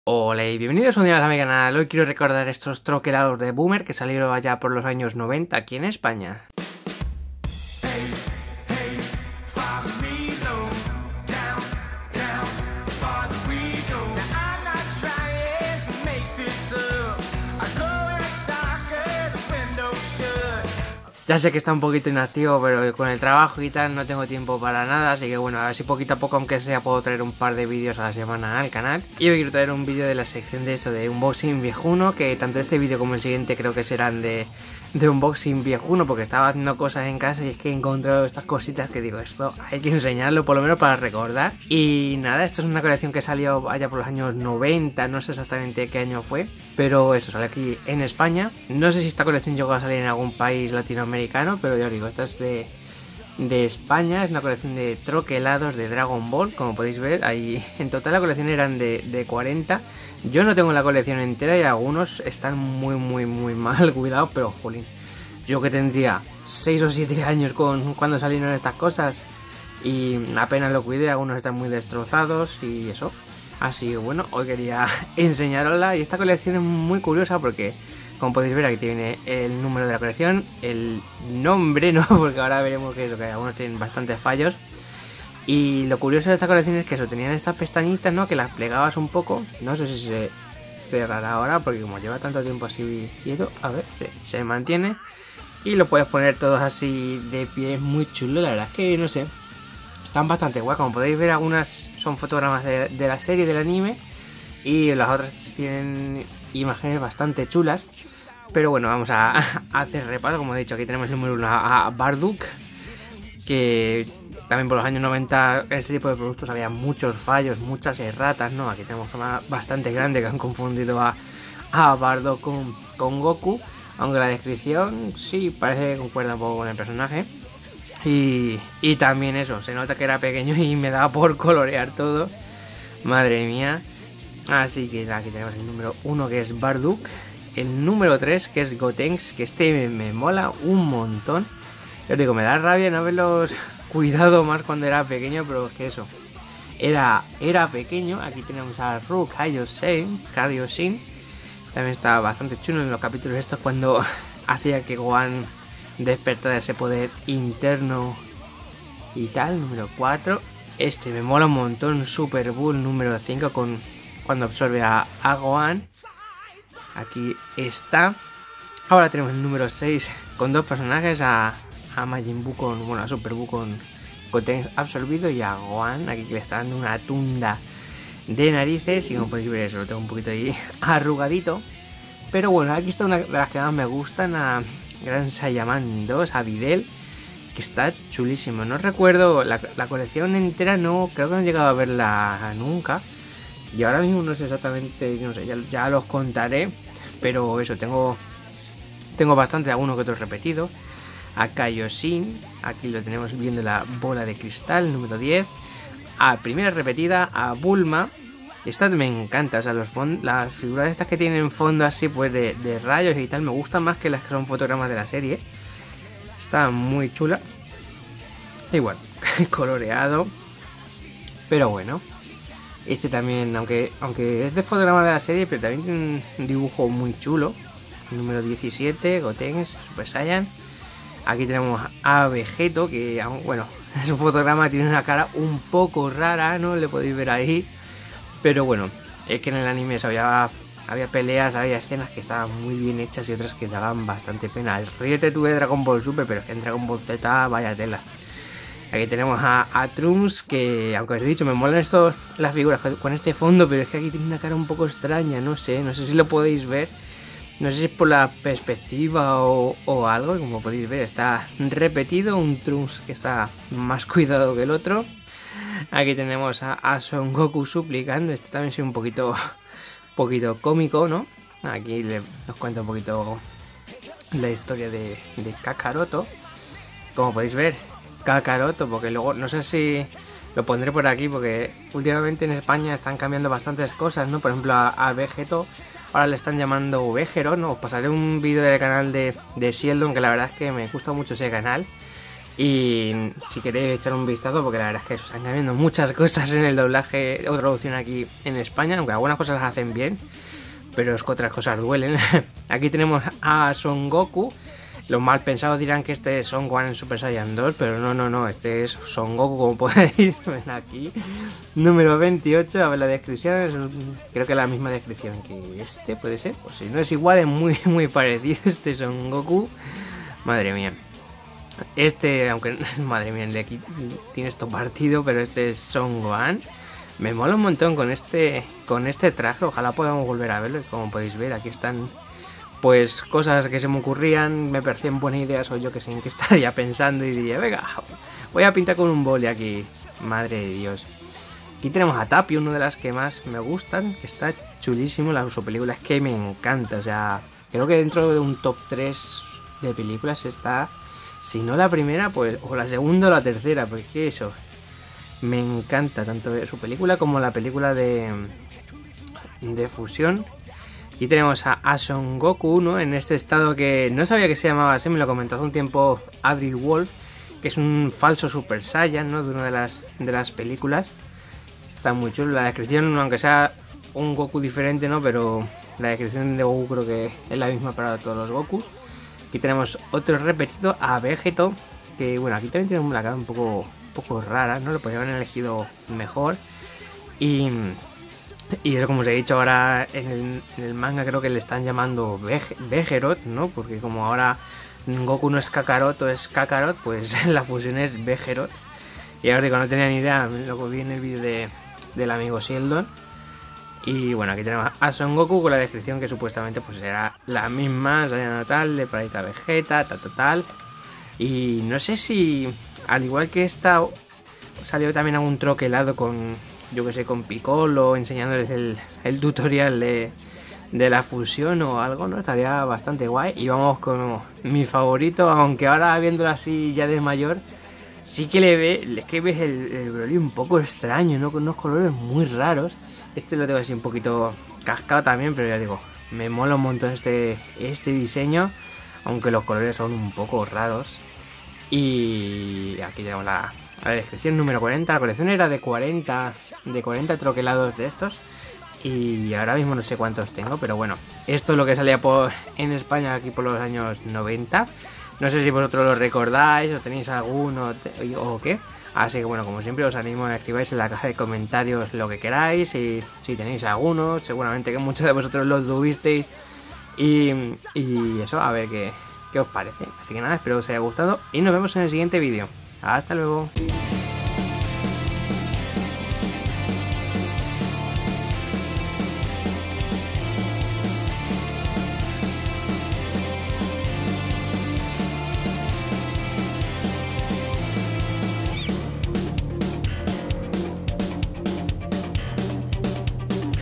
COLECCIÓN DE LOS AÑOS 90 - TROQUELADOS DRAGON BALL DE BOOMER - JESUSNOID - UNBOXING VIEJUNO
Hola y bienvenidos un dia mas a vuestro canal :D

Hoy traigo el unboxing viejuno o repaso de esta colección que salió allá por los años 90 ^^ era pequeño y algunos troquelados no los cuidé muy bien pero bueno, quería compartirlo por aquí ^^